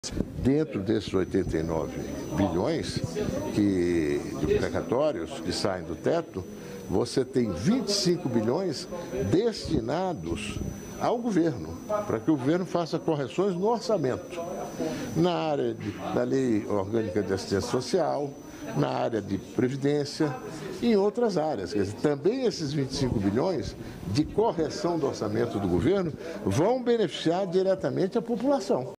PEC dos Precatórios: Senadores entregam proposta alternativa
Parlamentares José Aníbal (PSDB-SP), Alessandro Vieira (Cidadania-SE) e Oriovisto Guimarães (Podemos-PR) entregaram texto paralelo ao da PEC dos Precatórios aprovado na Câmara dos Deputados.